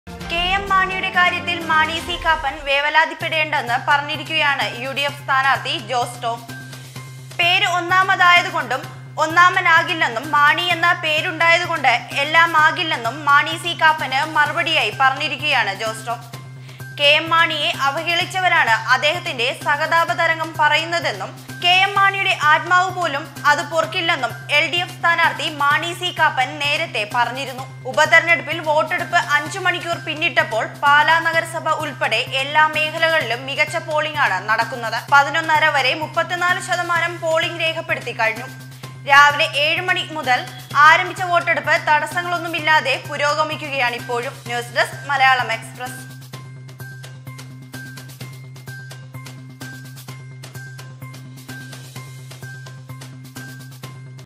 മാണി സി കാപ്പന് മറുപടിയുമായി ജോസ് ടോം.പാലാ പോര് മുറുകുന്നു
MalayalamExpressNews മാണി സി കാപ്പന് മറുപടിയുമായി ജോസ് ടോം.പാലാ പോര് മുറുകുന്നു